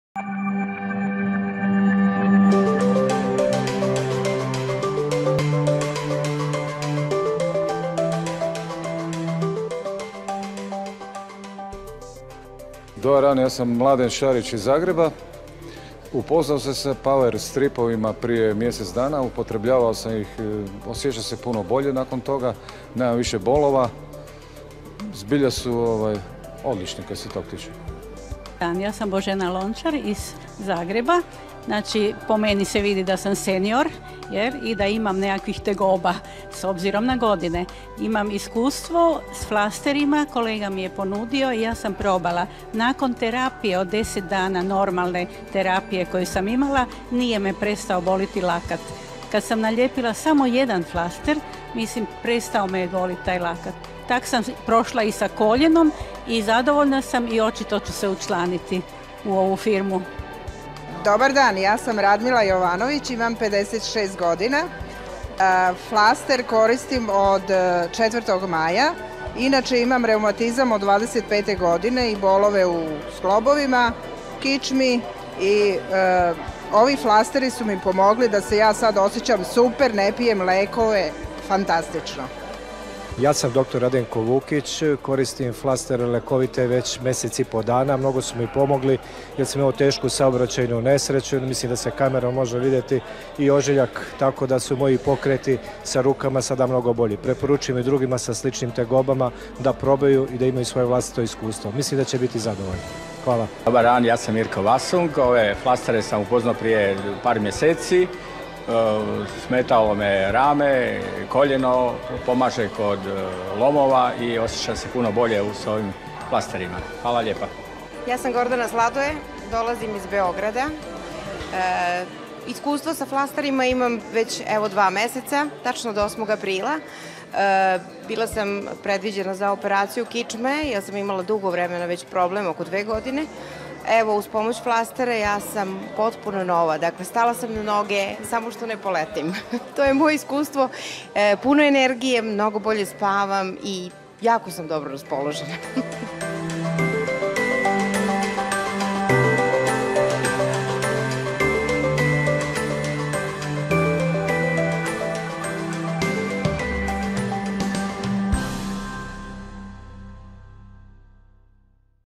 Gerincműtét helyett alkalmazott PowerStrips tapasz
A tapasz sajátossága az FDA (Food and Drug Administration - USA) engedély és a Clas 1. besorolás, mint gyógyászati segédeszköz.
A tapaszról tájékozódjon a oldalon.